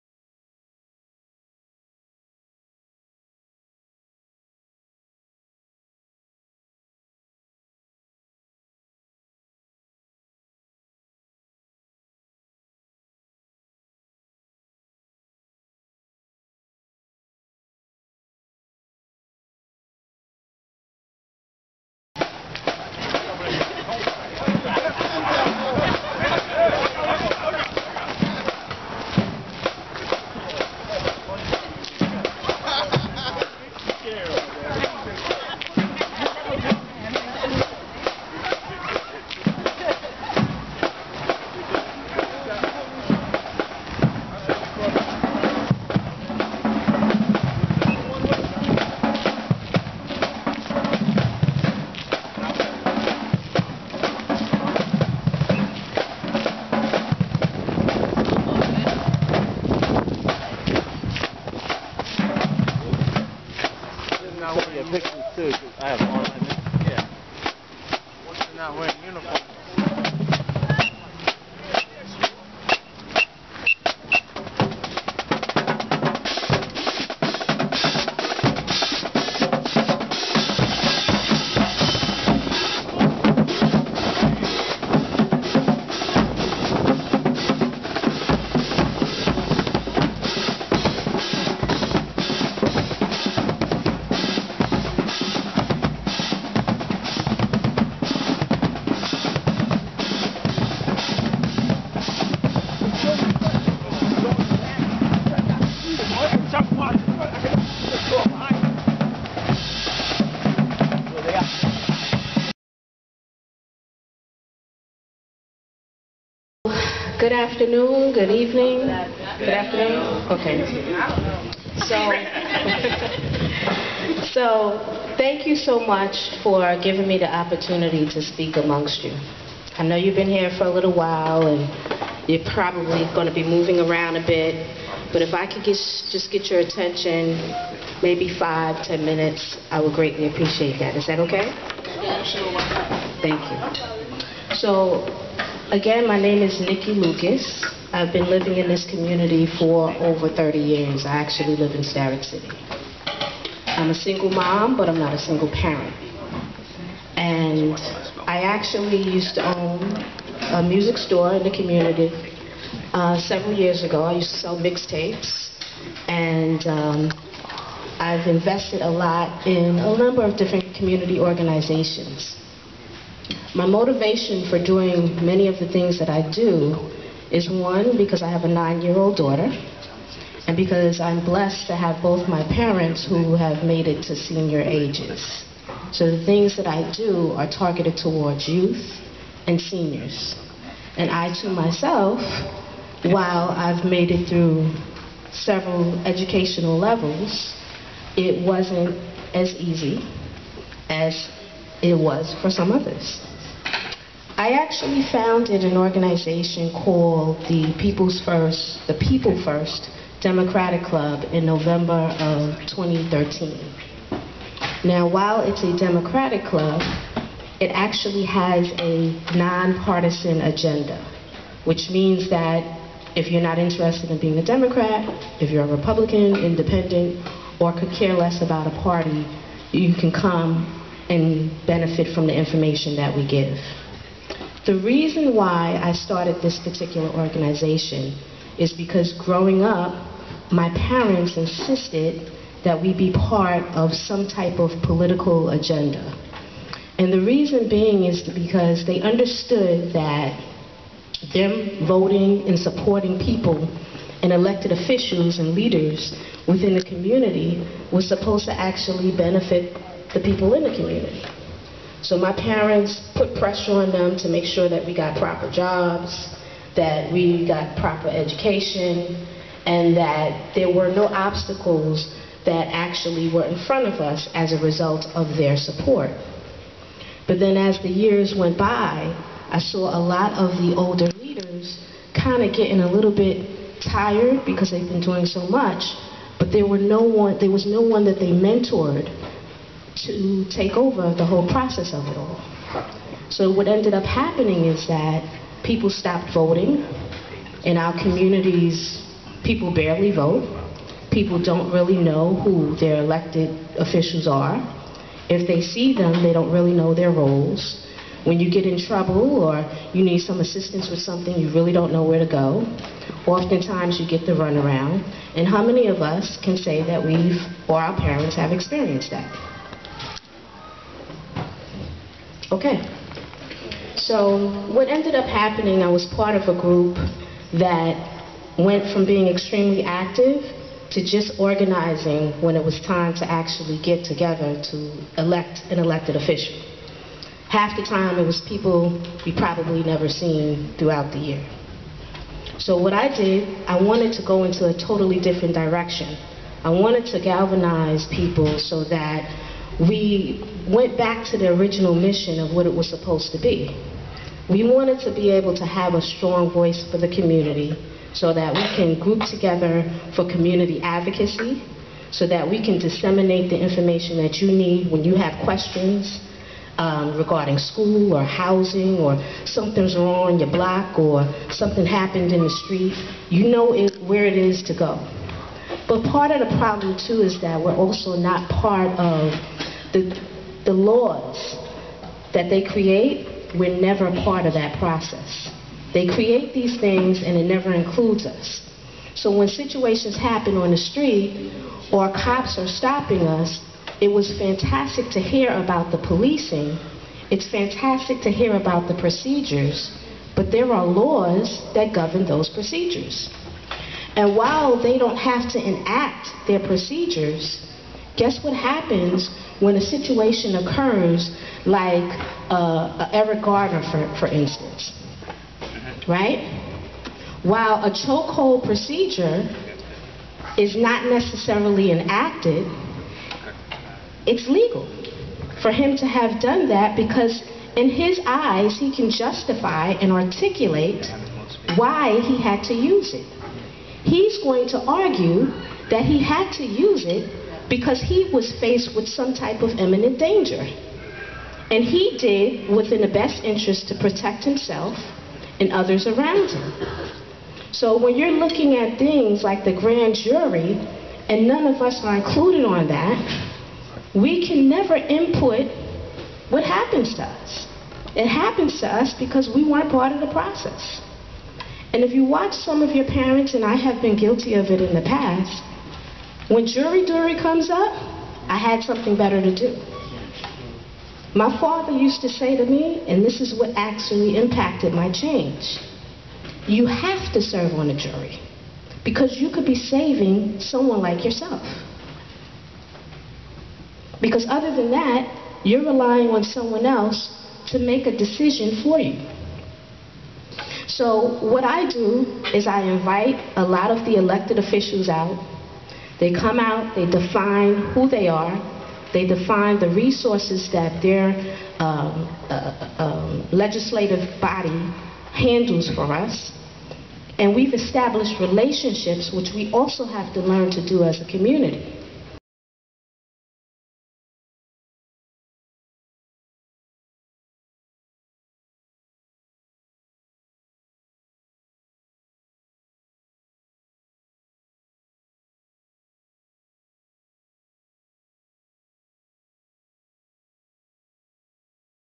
75th Precinct NYPD and ENYUYC Increase The Peace Stop The Violence Community Leader Presentation
This video is a presentation by East New York Community Leader Nikki Lucus. Her organization is People'sFirst Democractic Club (a non-partisan organization addressing the democratic process  and role of leaders in it. The presentation was part of the Increase The Peace/Stop The Violence March and Discussion held Feb 13 2015 in East New York Brooklyn by the 75th Precinct NYPD (Commander: Inspector Michael Lipetri) and East New York Urban Youth Corps (CEO Winchester Key)